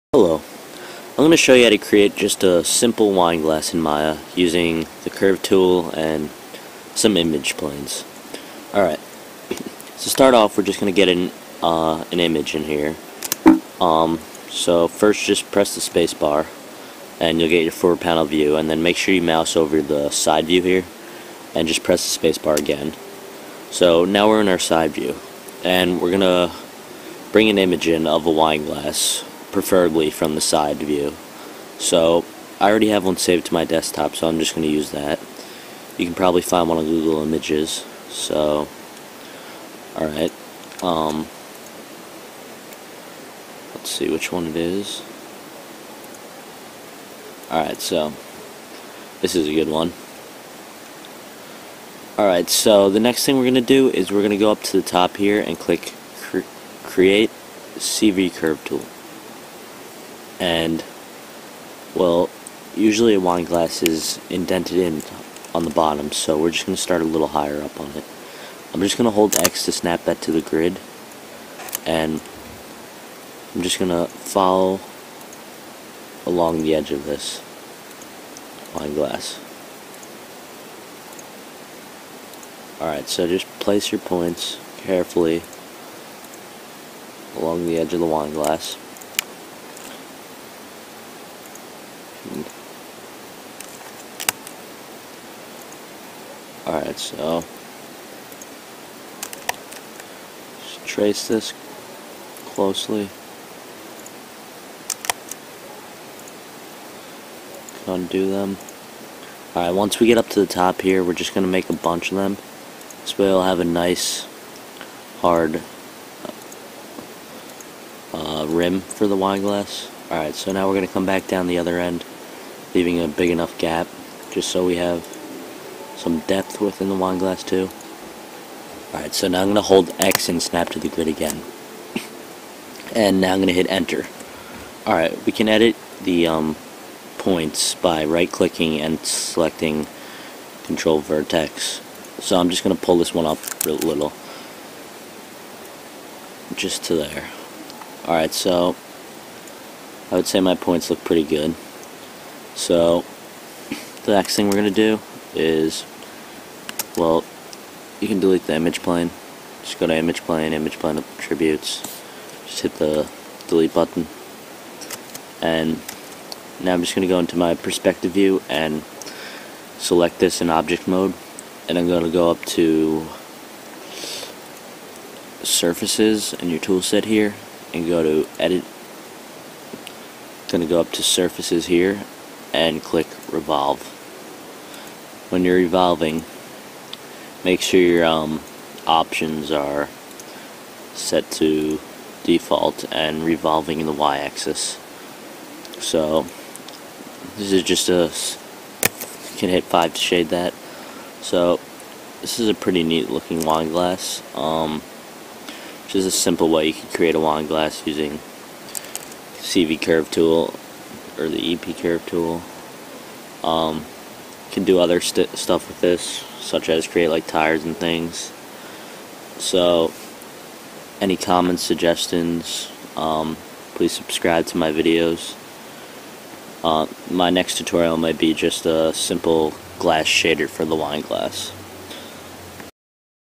Maya Tutorial - How to Model a Wine Glass
create a wineglass using the curve tool